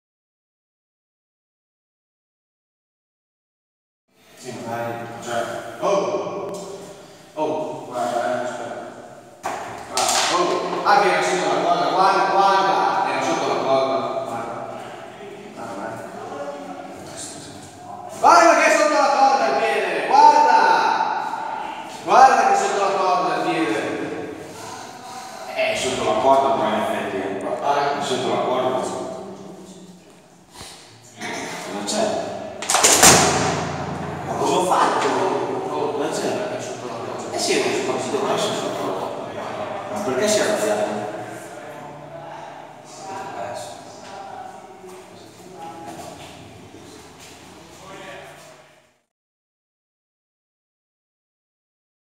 Salvataggio in corner per Nick Lenders?
Nick Lenders è di pessimo umore dopo aver perso il Titolo Italiano di Wrestling contro Andy Manero lo scorso sabato a ICW Colpo Finale 2018. Ma è andata veramente così?

Tony Callaghan e Riot sono convinti del contrario, e mostrano al Fuoriclasse un video che potrebbe ribaltare le sorti dell'incontro che ha mandato in panchina il Goleador. Come andrà a finire? E come reagirà la Stella del Sabato Sera all'inevitabile ricorso dei Revolve?

Non perderti i prossimi sviluppi della vicenda sul Sito Ufficiale della ICW